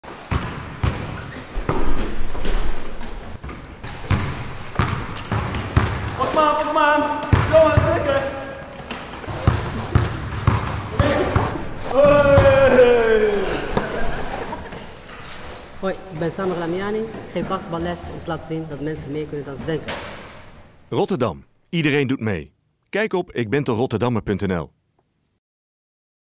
"Ik ben toch Rotterdammer!" (Samir)
De infomercial "Ik ben toch Rotterdammer!" voor Jeugd, Onderwijs en Samenleving, Gemeente Rotterdam. Deze participatiecampagne in het programma "Meedoen" en "Stadsburgerschap" is tevens ingezet op Abri's, trams, advertenties, neptattoos en in radiocommercials.